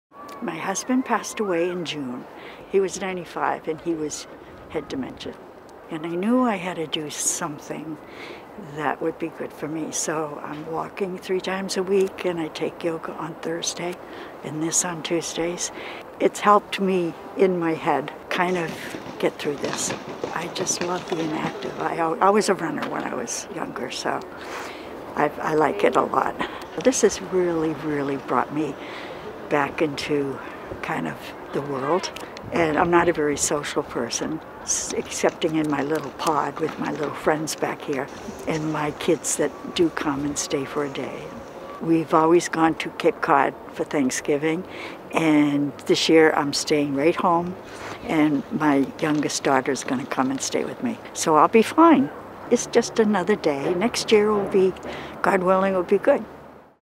Diane O'Hara
Newport senior Diane O'Hara stays safe and connected to the world through socially distanced boxing, yoga, and a small, local Thanksgiving.